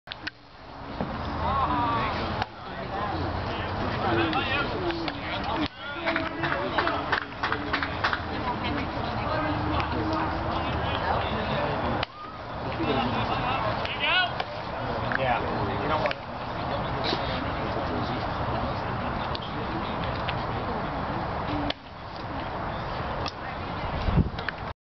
Henry Owens Warms Up
Edison High's Henry Owens (junior) tosses some warm-up pitches as he enters the game against JSerra Catholic.